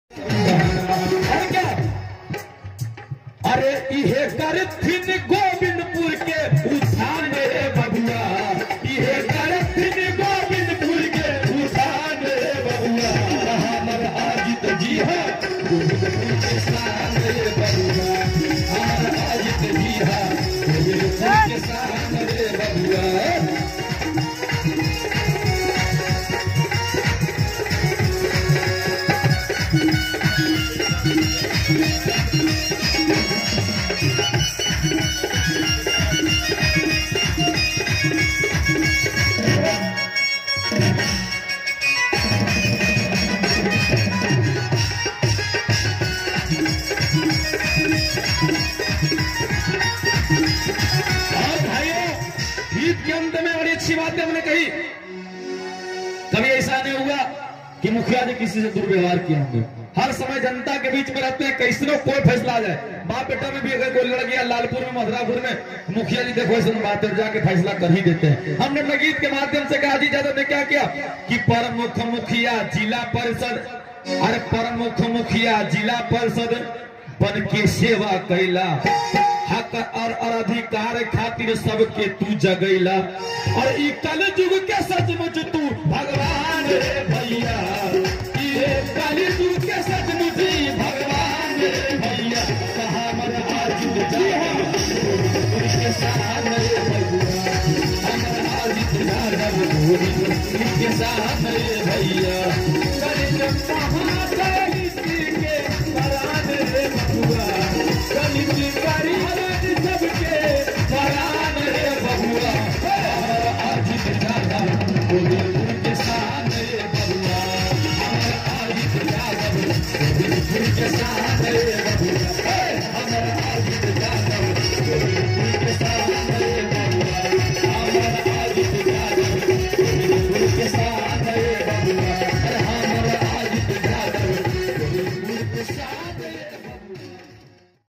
RJD Neta Bihar Pradesh mahasachiv Ajit Yadav kawakol Nawada Bihar
RJD अजीत यादव प्रदेश

महासचिव का अभिनंदन समारोह कौआकोल के ब्लॉक

में संबोधित कर रहे हैं पूरी कौवाकोल के जनता

गोविंदपुर विधानसभा रो कौआकोल नवादा बिहार